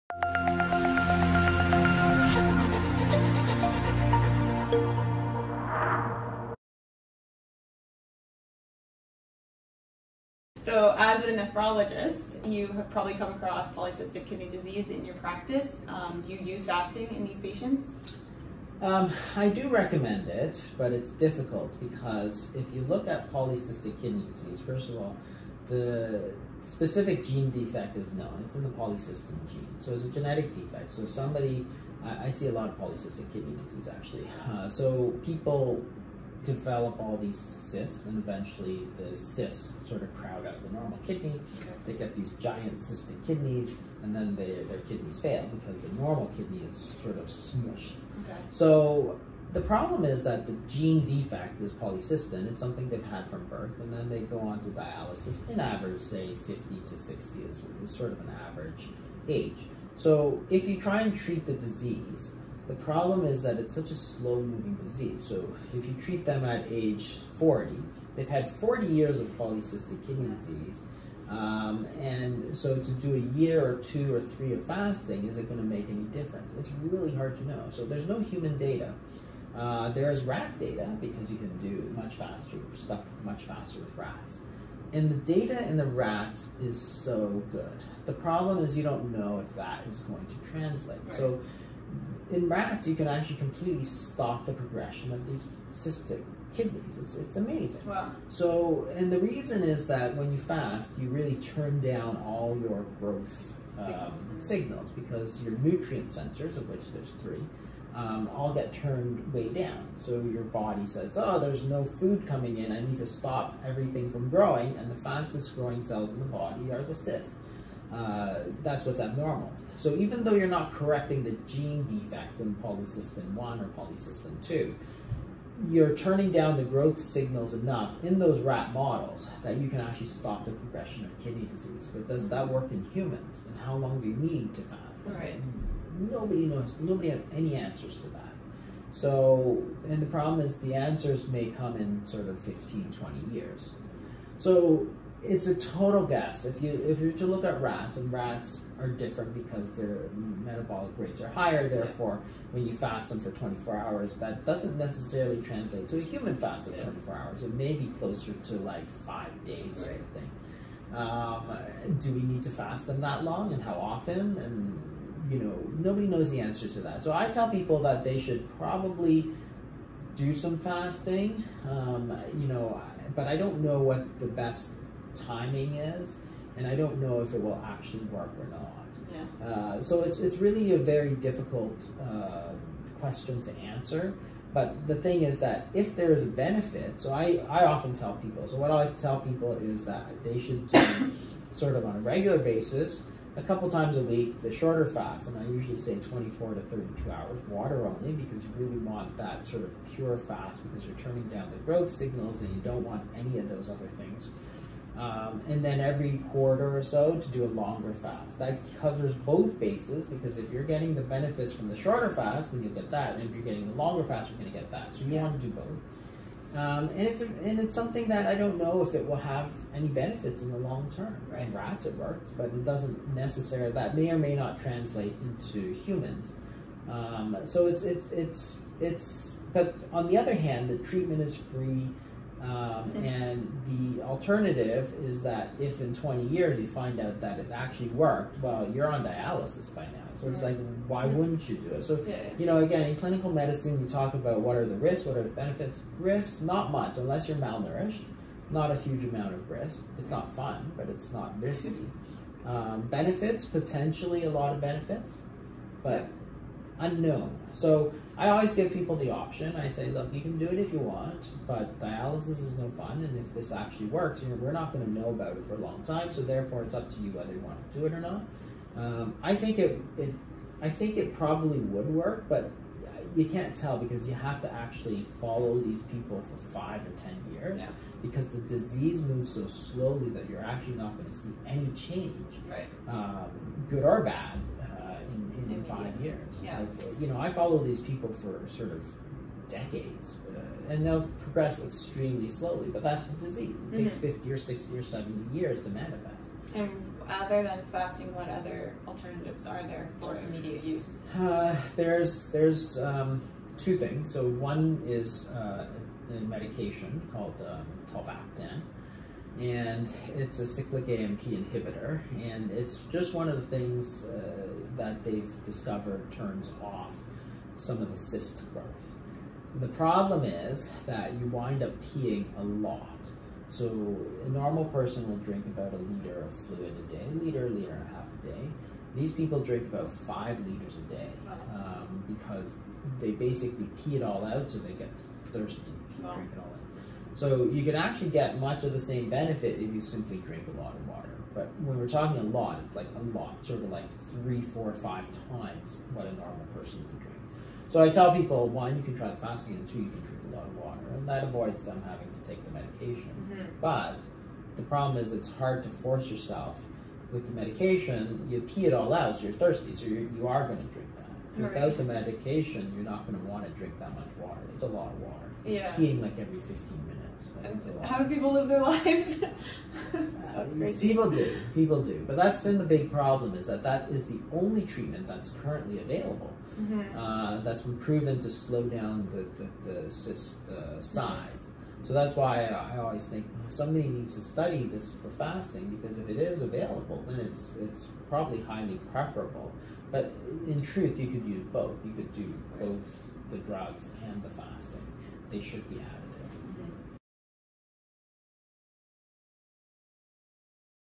Interview with Dr. Jason Fung nephrologist on polycystic kidney disease and fasting
Dr. Jason Fung is a Canadian nephrologist and a world-leading expert on intermittent fasting and low carb diets, especially for treating people with type-2 diabetes.